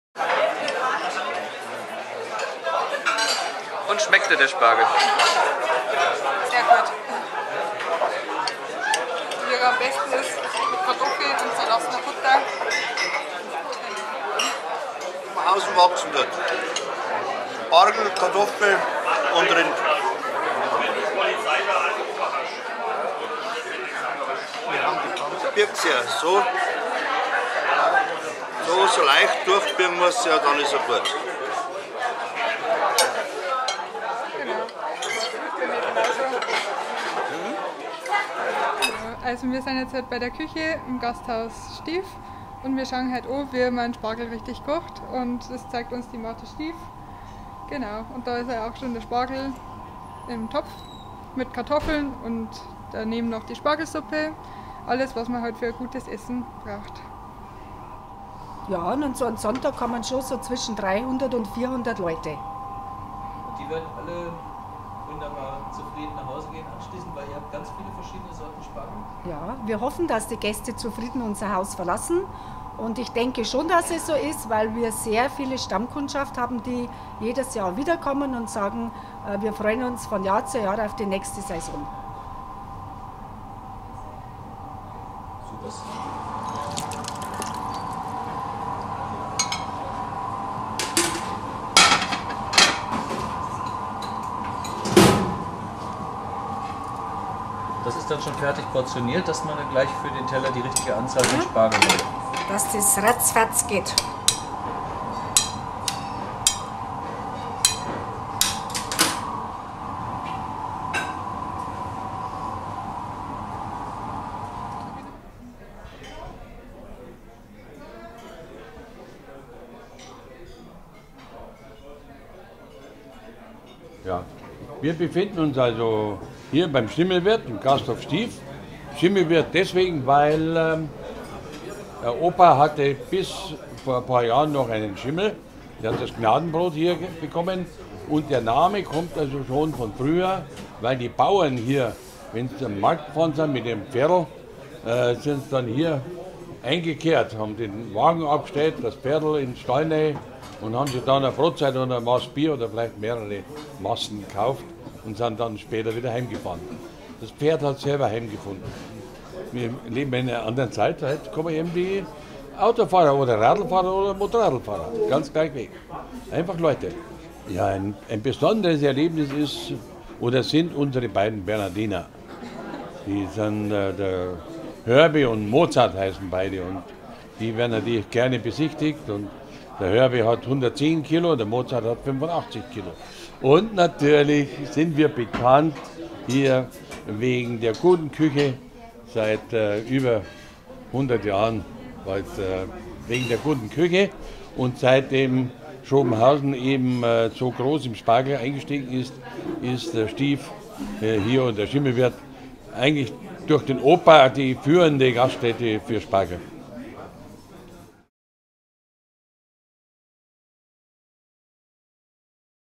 Spargel essen @ Metzgerei Jakob Stief "Zum Schimmelwirt" in Schrobenhausen
Spargel essen @ Metzgerei Jakob Stief "Zum Schimmelwirt" in Schrobenhausen. "Beim Stief" bzw. "beim Schimmelwirt" trifft man sich nicht nur zur Spargelzeit in Schrobenhausen: weit über den Ort hinaus bekannt ist das in 4. Generation betriebene Gasthaus & Metzgerei Jakob Stief "Zum Schimmelwirt".  Hier drängen sich dann zur Spargelzeit Auto- und Bustouristen vor Tellern mit dem weißen Edelgewächs, das hier in diversen Variationen auf der Speisekarte steht.